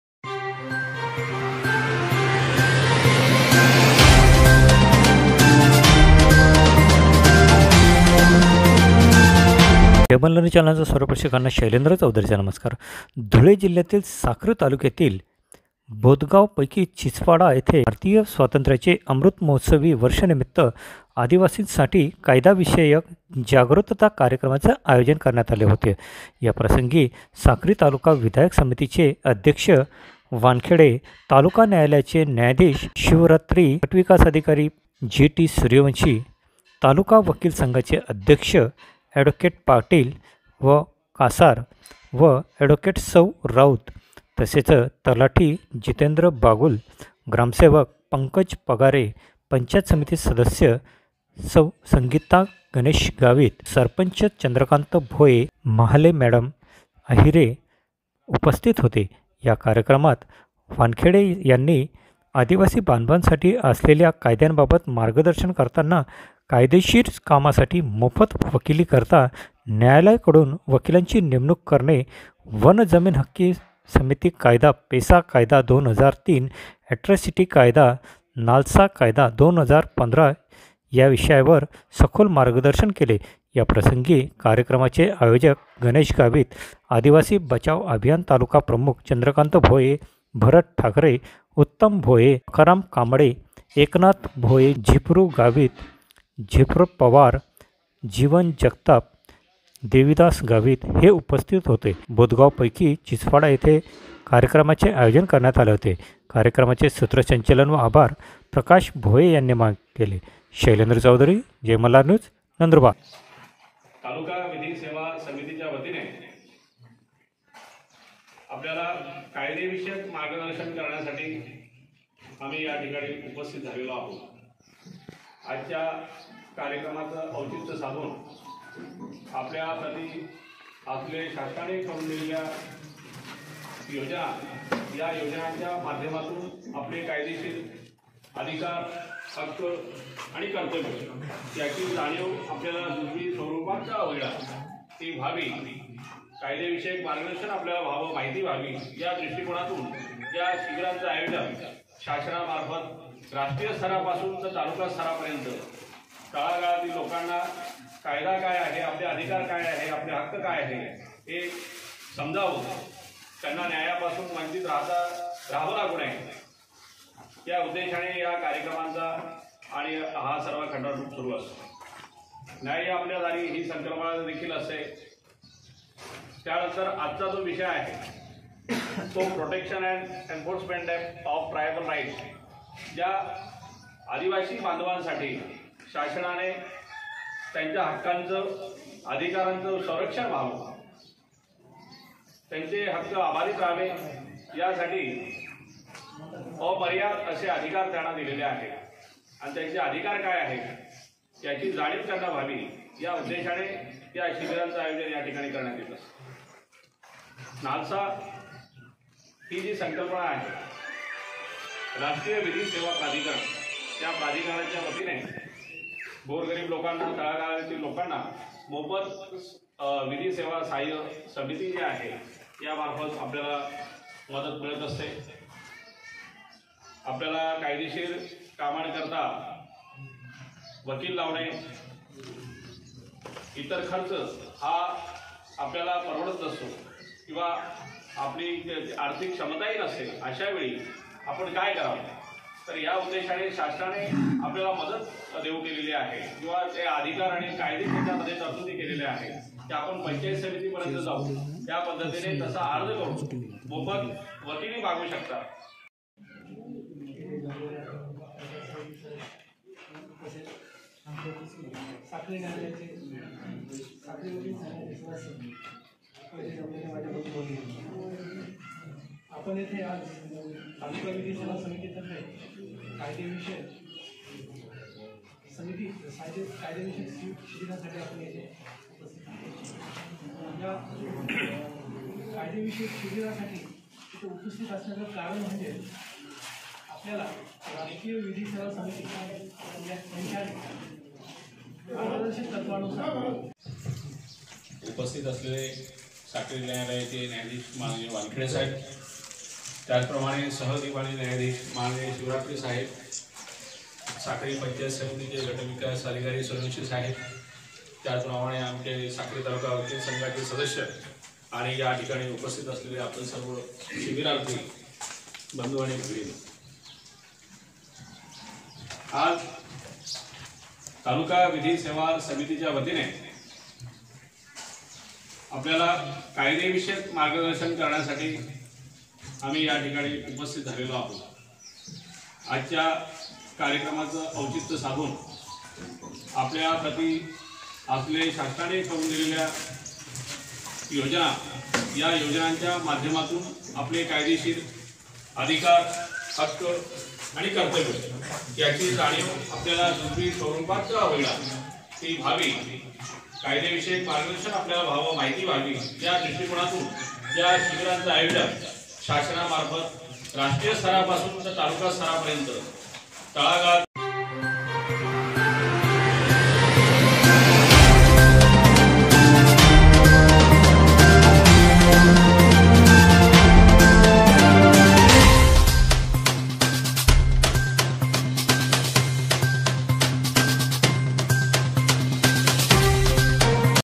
चिंचपाडायेथे भारतीयस्वातंत्र्याचेअमृत महोत्सवीवर्षानिमित्तआदिवासिंसाठी कायदाविषयक जागृतताकार्यक्रम
बोदगाव येथे कायदेविषयक सल्ला मार्गदर्शन शिबिर.
बोदगाव पैकी चिंचपाडा येथे भारतीय स्वातंत्र्याचे अमृत महोत्सवी वर्षानिमित्त आदिवासी साठी कायदाविषयक जागृतता कार्यक्रमाचे आयोजन करण्यात आले होत.
याप्रसंगी साक्री तालुका विधी सेवा समिती चे अध्यक्ष वानखेडे,तालुका न्यायालयाचे न्यायाधीश शिवरात्रि,साक्री पंचायत समिती गटविकास अधिकारी जे.टी सूर्यवंशी,तालुका वकील संघाचे अध्यक्ष ॲड.पाटील,कासार व ॲड.सौ.राऊत तसेच तलाठी जितेंद्र बागुल,ग्रामसेवक पंकज पगारे, पंचायत समिती सदस्य सौ.संगीता गणेश गावित, सरपंच चंद्रकांत भोये,सौ.महाले,सौ.अहिरे उपस्थित होते.या कार्यक्रमात न्यायाधीश वानखेडेंनी आदिवासी बांधवांसाठी असलेल्या कायद्यांबाबत मार्गदर्शन करताना कायदेशीर कामासाठी मोफत वकिली करता न्यायालयाकडून वकिलांची नेमणूक करणे,वनजमीन हक्क समिती कायदा,पेसा कायदा 2003,ॲट्रासिटी कायदा,नालसा कायदा 2015 या विषयावर सखोल मार्गदर्शन केले याप्रसंगी कार्यक्रमाचे आयोजक गणेश गावित आदिवासी बचाव अभियान तालुकाप्रमुख चंद्रकांत भोये,भरत ठाकरे,उत्तम भोये,बकाराम कामडे,एकनाथ भोये झिपरू गावित,झिपरु पवार,जिवन जगताप, देविदास गावित हे उपस्थित होते.बोदगाव पैकी चिंचपाडा येथे कार्यक्रमाचे आयोजन करण्यात आले होते कार्यक्रमाचे सूत्रसंचालन व आभार प्रकाश भोये यांनी मानले.